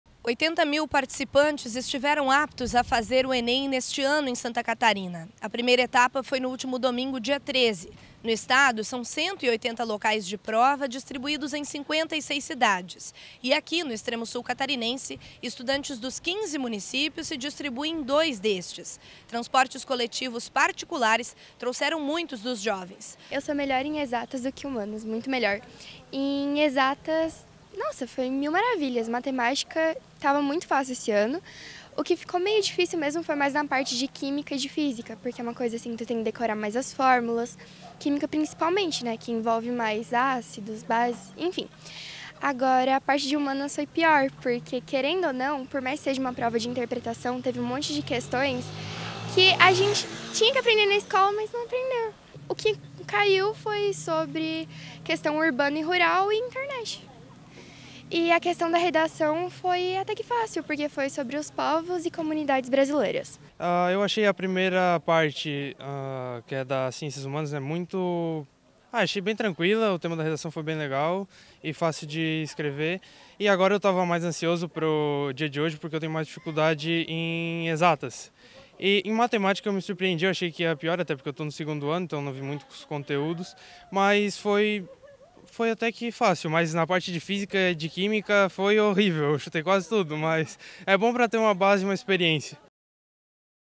21/11/22 ENEM: 2ª ETAPA FOI REALIZADA NO ÚLTIMO DOMINGO COM LOCAIS DE PROVA EM ARARANGUÁ E SOMBRIO
Reportagem: Sofia Dessuy
Data: 21/11/22